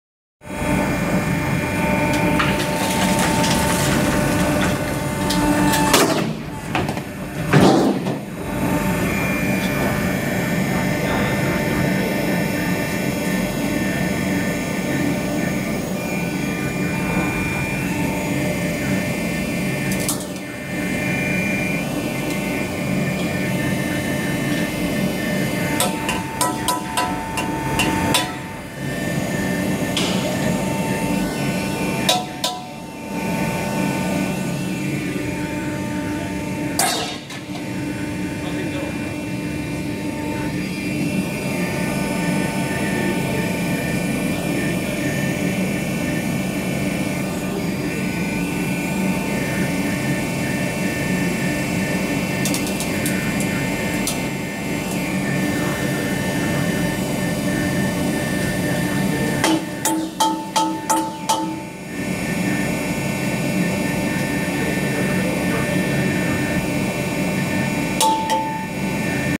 Bronneberg Catalytic Converter alligator shear - Katalysator Recycling Alligatorschere
This Bronneberg alligator shear is designed to cut Catalytic converters in order to take out the monolith. Monolith contains precious metals such as platinum, rhodium and palladium.

Alligatorschere zum schneiden von Katalysatoren.

Cisailles crocodiles pour recyclage des pots pots catalytiques.

NOŻYCE ALIGATOROWE DO CIĘCIA KATALIZATORÓW
BRONNEBERG
Te nożyce aligatorowe są skonstruowane specjalnie do cięcia katalizatorów, do odzyskania monolitu. Monolit zawiera cenne metale takie, jak: platyna, rod i pallad.